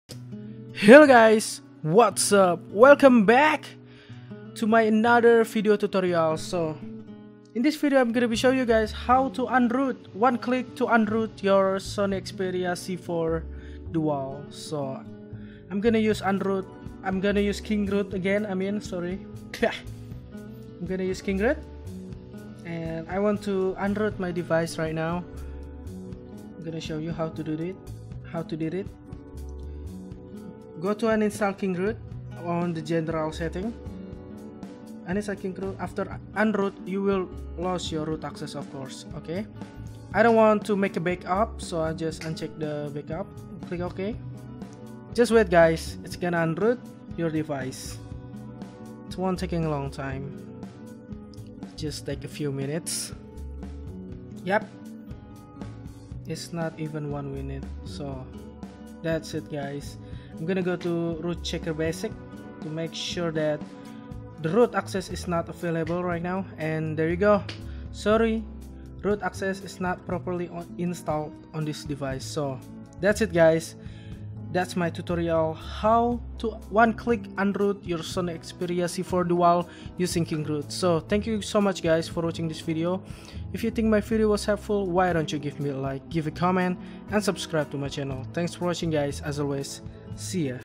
One Click Unroot Any Android Devices (Using Kingroot)
Back With Another Video Tutorial, So on this video i'm gonna show you how to ONE CLICK UNROOT ANY ANDROID DEVICES (Using Kingroot)